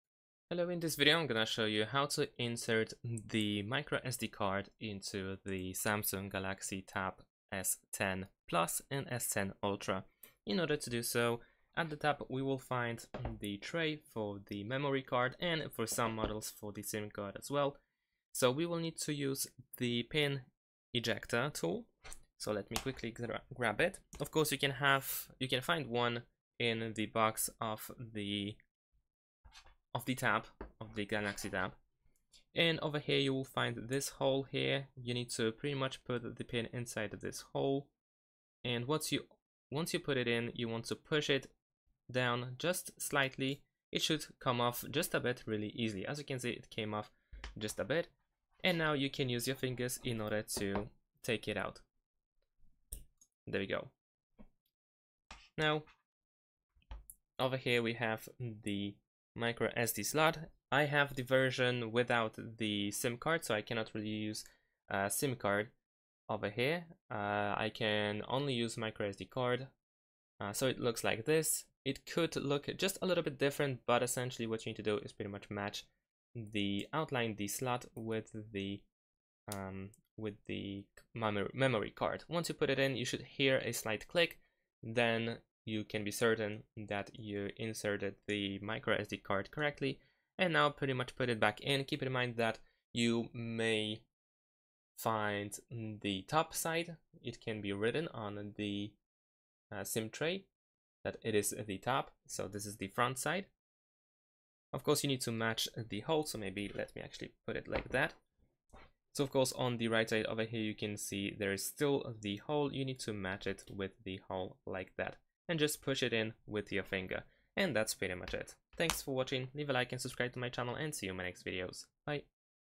Samsung Galaxy Tab S10+/S10 Ultra: How to Insert SD Card - Insert microSD Memory Card
In this video, we guide you through the process of inserting an SD card into your Samsung Galaxy Tab S10+ or S10 Ultra. Learn the steps to properly insert a microSD memory card for expanded storage options, allowing you to save more apps, photos, and files. We’ll provide tips on how to format the card for optimal use and access your stored content easily. Maximize your tablet's storage capabilities with this straightforward tutorial!